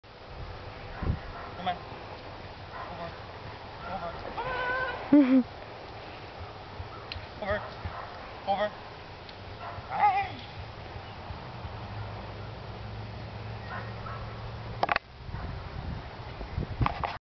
Agilité et Kong
Jean-Yves entraîne Truman par le jeu